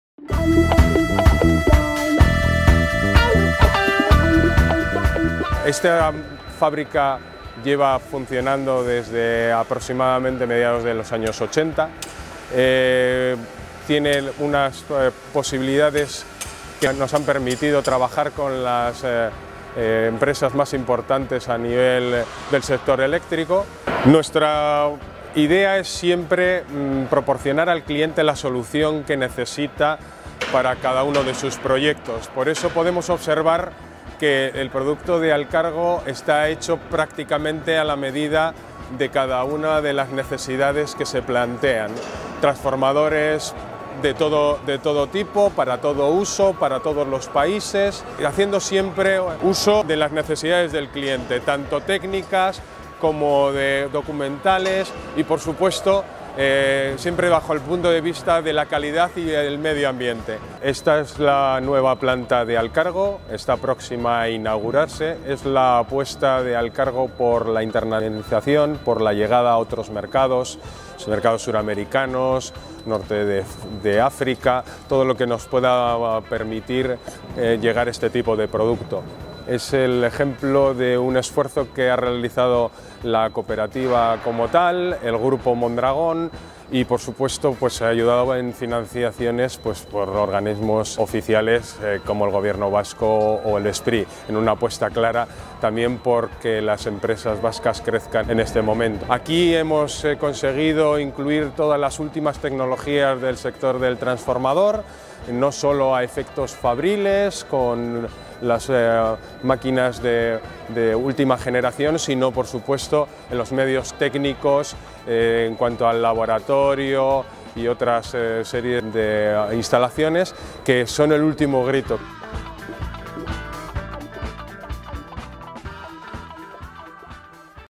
Nueva planta de Alkargo que dispone en sus instalaciones de varias grúas GH.
Alkargo invierte 17 millones de euros en una nueva planta de transformadores. Cuenta en sus instalaciones con varias grúas puente de GH Cranes & Components.